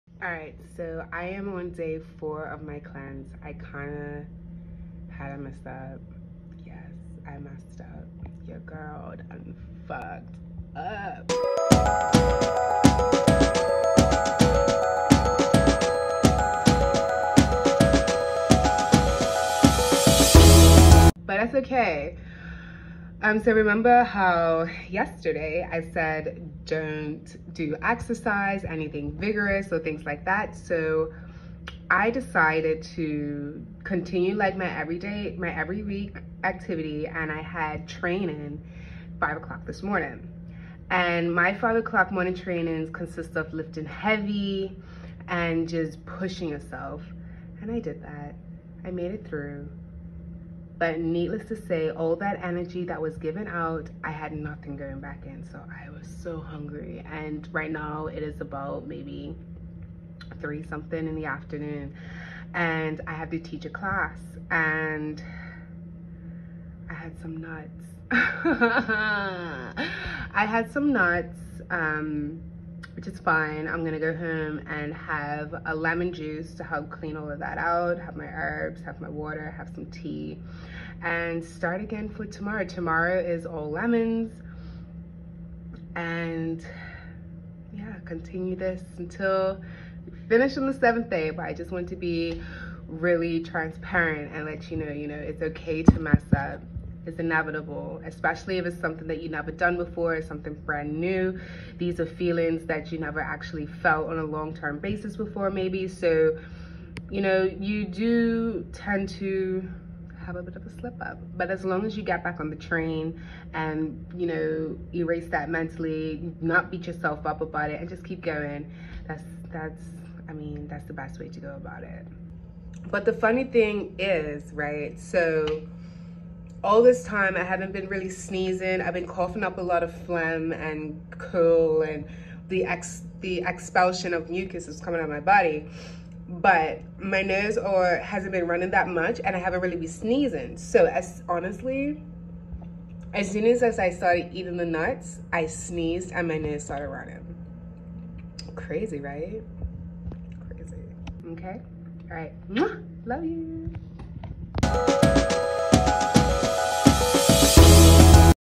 I F*cked up !!!!
So I cheated on my cleanse and I wanted to be super transparent.
I had Nut and now my stomach feels weird and my nose is runner and I’m sneezing .. mind you these nuts had additive to them that were overly sweet so I think the sugar messed me right up.

But any whoooo back on it